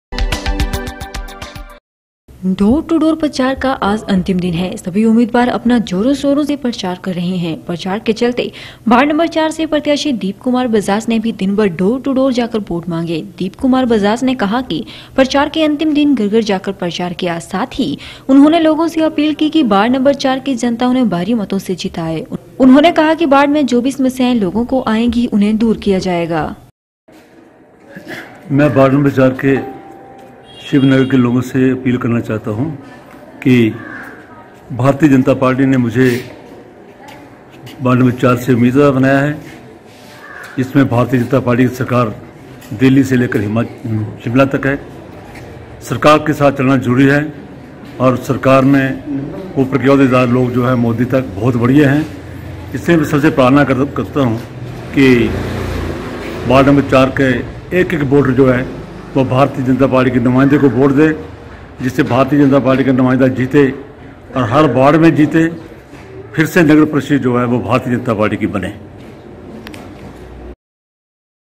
8 JAN 15 वार्ड नंबर 04 से प्रत्याशी दीप कुमार बजाज का डोर टू डोर प्रचार अतिंम दिन भी जारी
डोर टू डोर प्रचार का आज अतिंम दिन है सभी उम्मीदवार अपना जोरो शोरों से प्रचार कर रहे है प्रचार के चलते वार्ड नंबर चार से प्रत्याशी दीप कुमार बजाज ने भी दिन भर डोर टू डोर जाकर वोट मांगे है। दीप कुमार बजाज ने कहा कि प्रचार के अतिंम दिन घर घर जाकर प्रचार किया है साथ ही उन्होनें लोगों से अपील की वार्ड नं0 चार की जनता उन्हें भारी मतों से जिताए उन्होनें कहा कि वार्ड में जो भी समस्याएं लोगों को आऐगी उन्हें दूर किया जाएगा ।